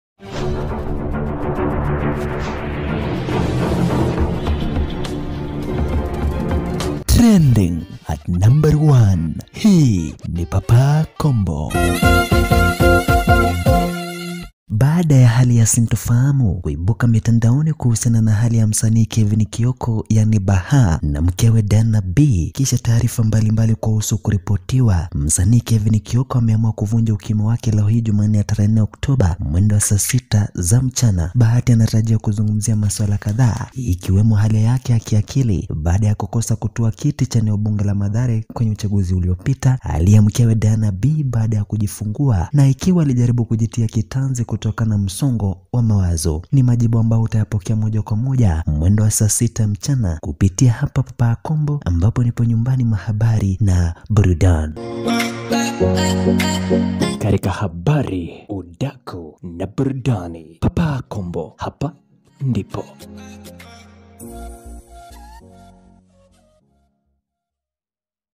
Finally!:BAHATI & DIANA B SPEAKS ON THEIR SITUATION!: WHAT HAS BEEN HAPPENING BETWEEN THEM
WHAT HAS BEEN GOING ON BETWEEN THE BAHATIS